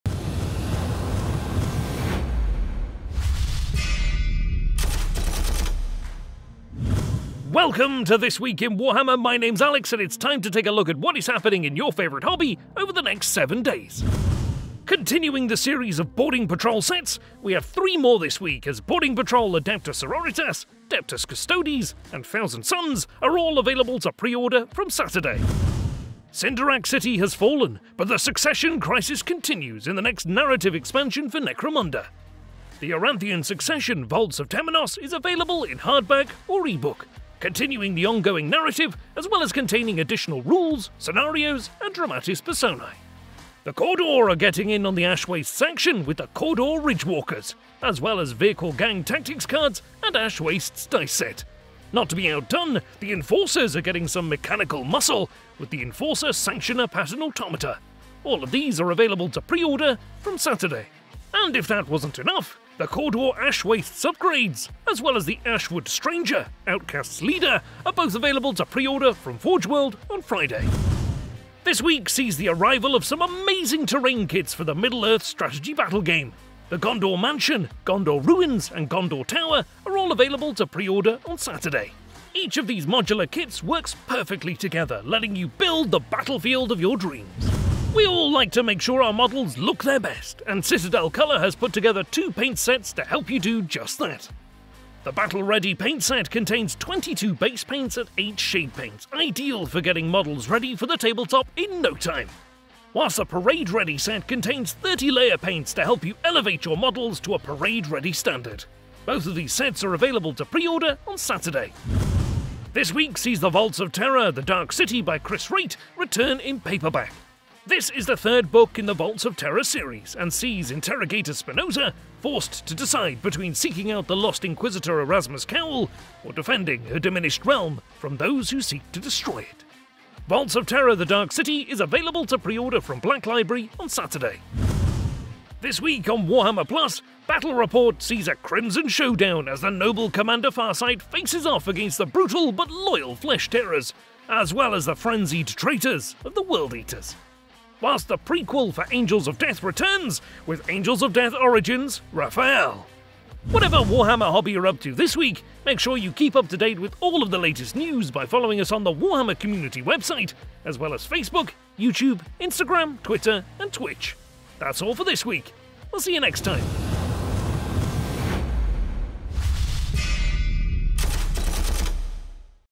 This Week in Warhammer: Great Upheavals in the Underhive
New Boarding Patrols, political intrigue on Necromunda, and more – check out this week's Warhammer highlights.